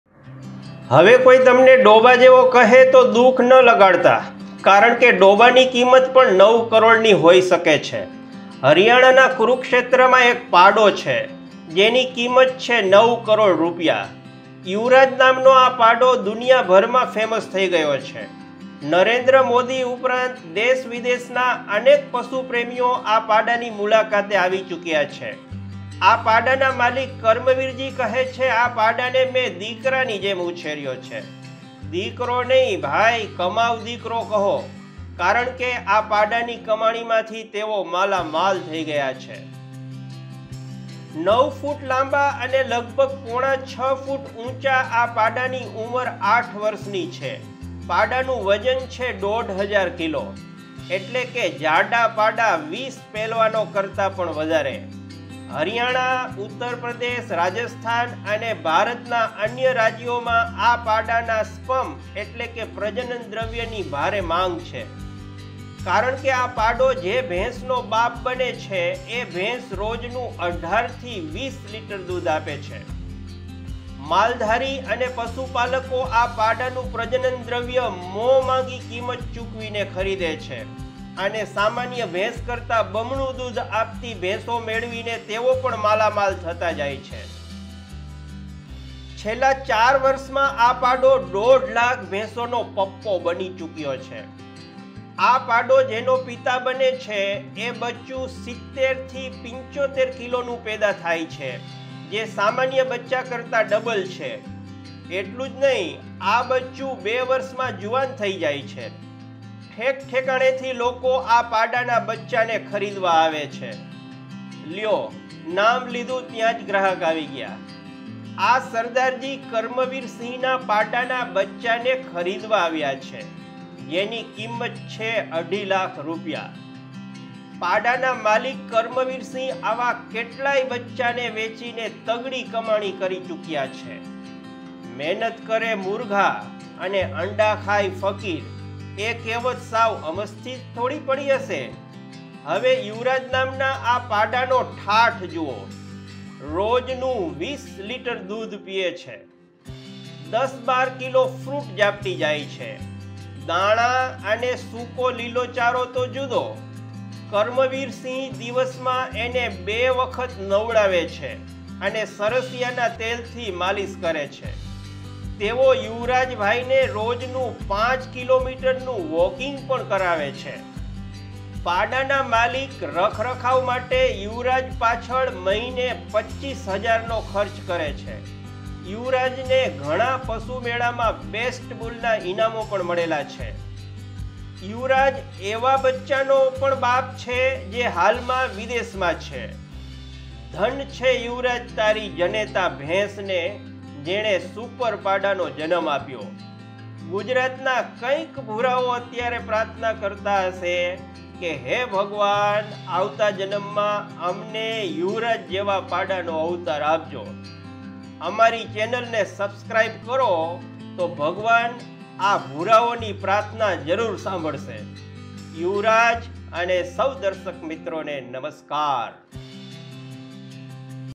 Yuvraj The Super Bull | 9 કરોડનો પાડો યુવરાજ
Yuvraj is no ordinary bull. Its seed fetches its owner Rs 150,000 for one ejaculation. The 9-year-old bull of Murrah breed drinks 20 litres of milk every day to stay fit and healthy. He has so far fathered more than 200,000 calves, his owner says. Check out the video to see the fun facts about Yuvraj the Super Bull.